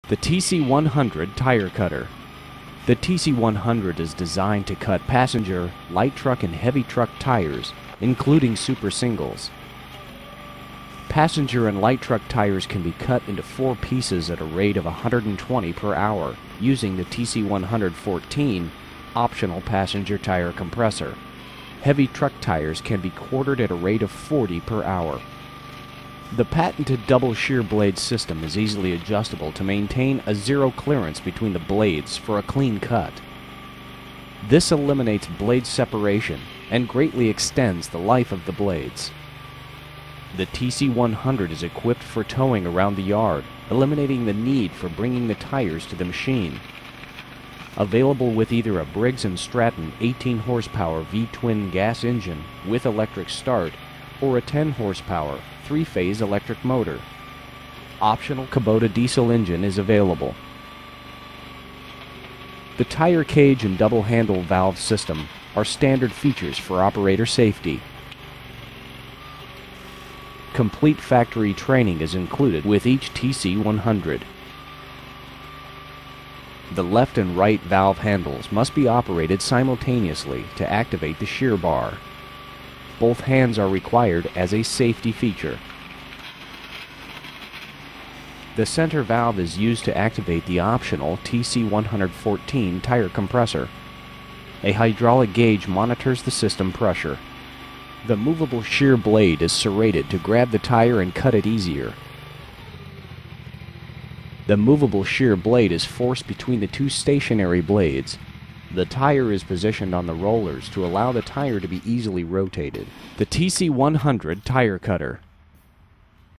TC-100 Tire Cutter
The TC-100 Tire Cutter is designed to cut passenger, light truck and heavy truck tires; including super singles. passenger and light truck tires can be cut into 4 pieces at a rate of up to 120 per hour using the optional passenger tire compressor.